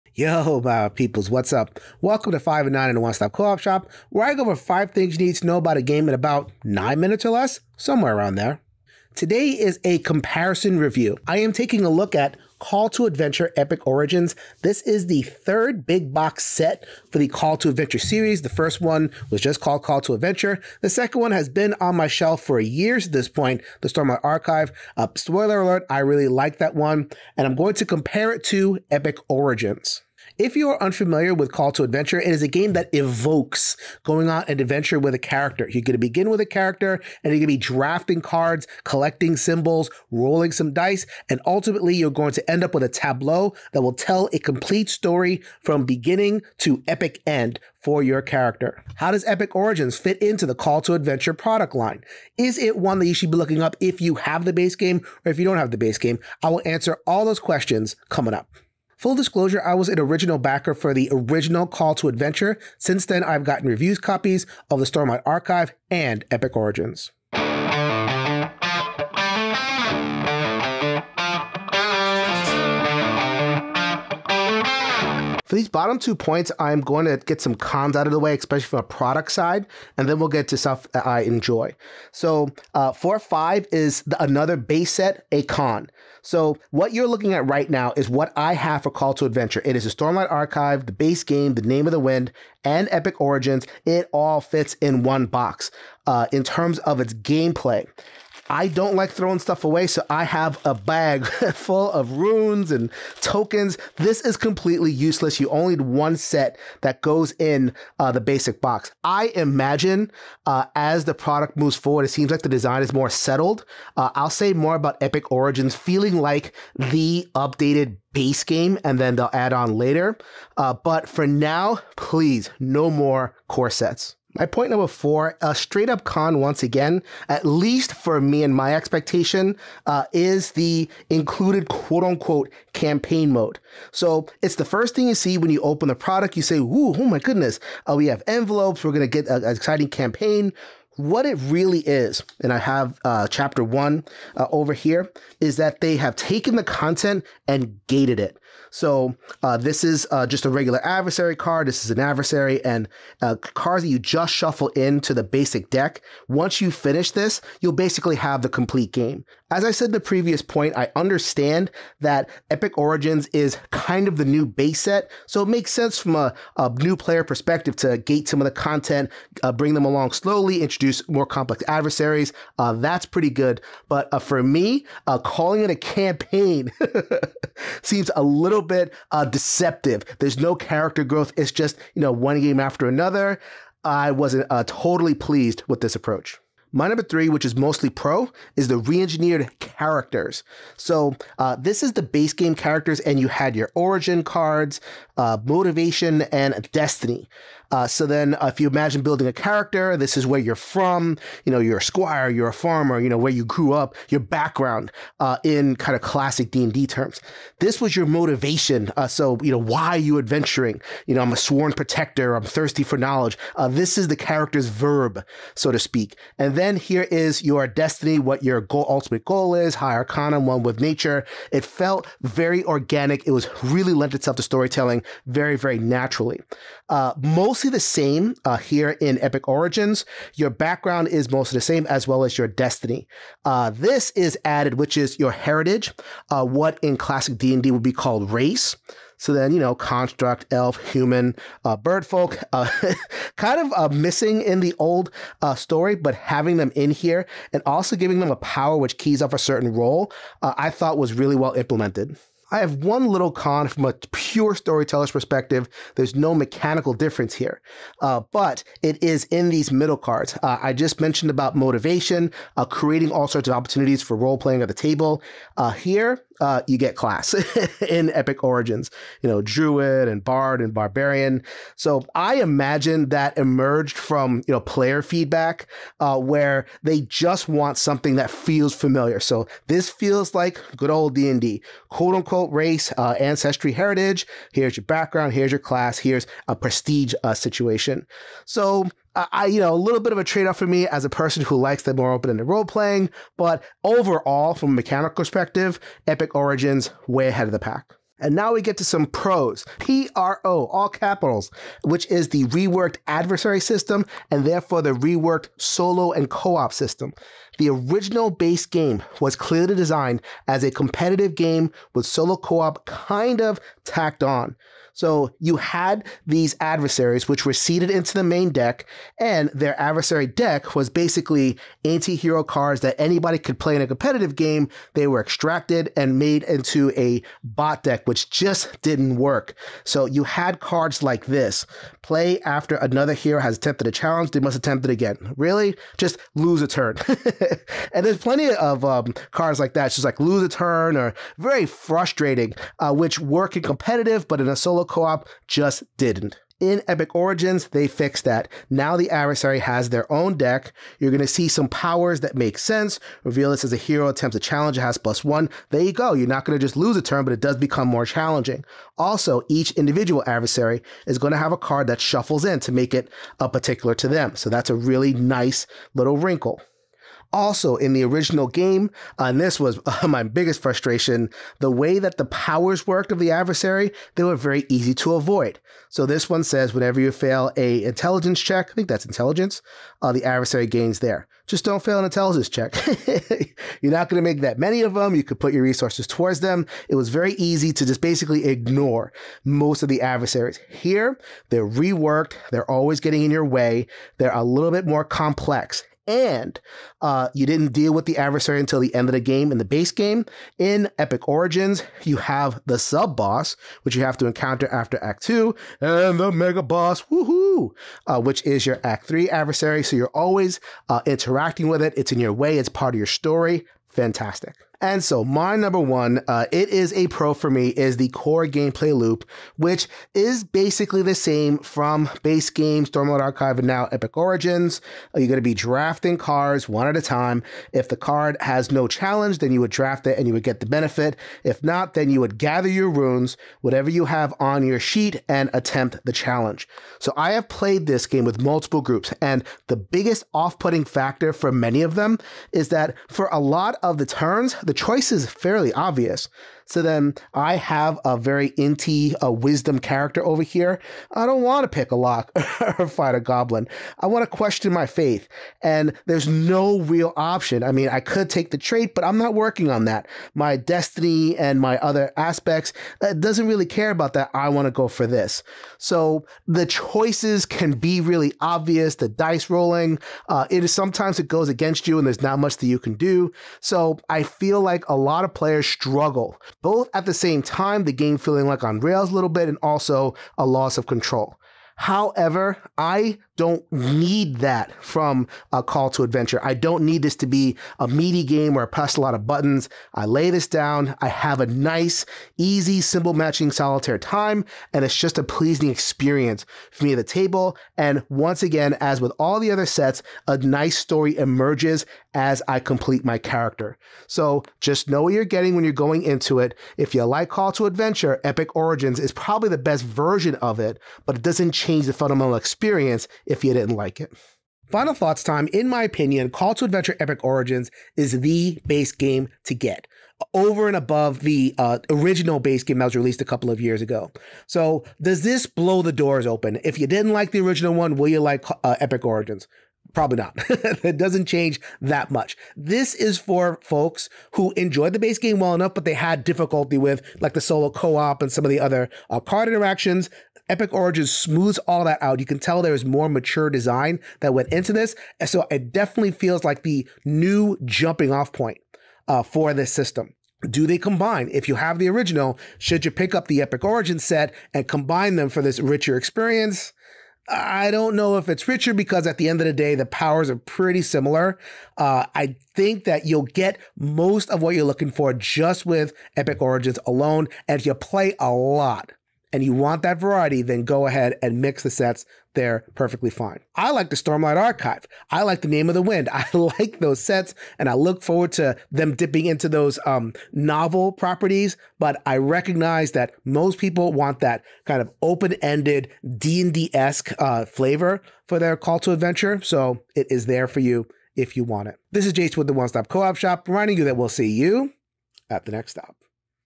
Call to Adventure: Epic Origins | Review | With Jason
Jason compares the newest Call to Adventure release to the original.
0:00 - Introduction
1:15 - Another Core Set
2:04 - "Campaign" Structure
3:06 - Tweaked Characters
4:58 - Reworked Solo/Co-Op
6:54 - Core Gameplay Loop
8:34 - Final Thoughts